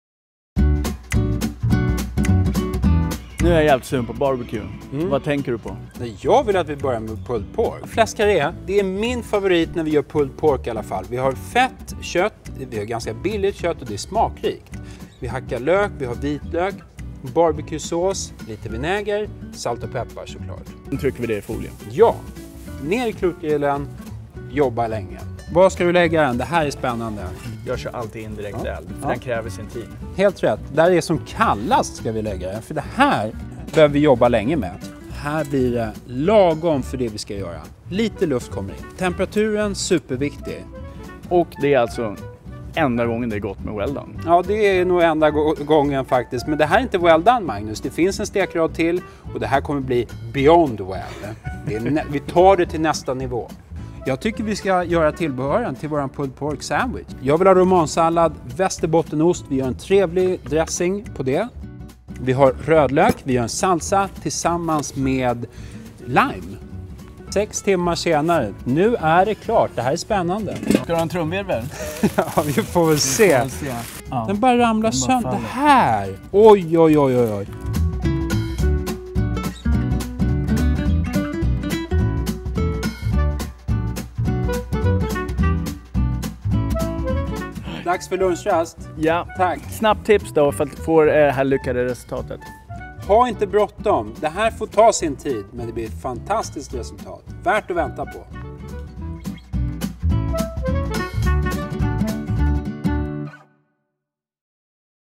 Pulled Pork Sandwich Opskrifter | Unilever food solutions Dansk
Magnus Rydberg (BBQ-verdensmester) og Jonas Ålund lærer dig at mestre den ægte American Grill ved at tilberede røget oksespidsbryst med grillet majs og citron